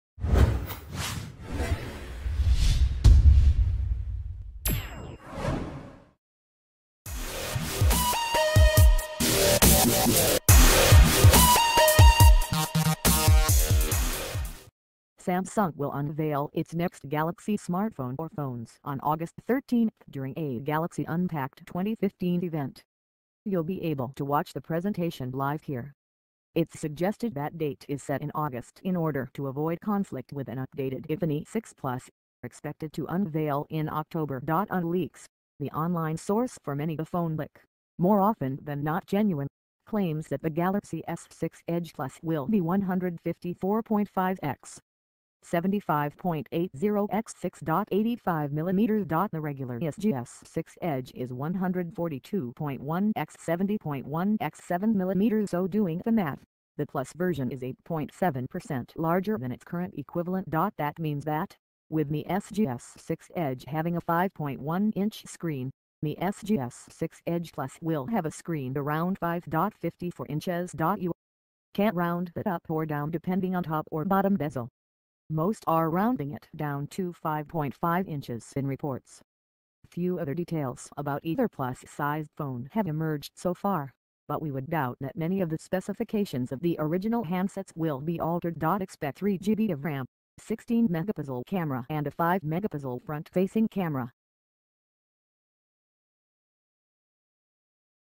Samsung Galaxy S6 Edge Plus New Smartphone First Look ᴴᴰ
Samsung will unveil its next Galaxy smartphone or phones on 13 August during a "Galaxy Unpacked 2015" event. You'll be able to watch the presentation live here. It's suggested that date is set in August in order to avoid conflict with an updated iPhone 6 Plus, expected to unveil in October.OnLeaks, the online source for many a phone leak - more often than not genuine - claims that the Galaxy S6 edge Plus will be 154.5 x 75.80 x 6.85mm. The regular SGS6 edge is 142.1 x 70.1 x 7mm so doing the math, the Plus version is 8.7 per cent larger than its current equivalent.That means that, with the SGS6 edge having a 5.1-inch screen, the SGS6 edge Plus will have a screen around 5.54-inches. You can round that up or down depending on top or bottom bezel. Most are rounding it down to 5.5-inches in reports. few other details about either Plus-sized phone have emerged so far, but we would doubt that many of the specifications of the original handsets will be altered.Expect 3GB of RAM, 16-megapixel camera and a 5-megapixel front-facing camera.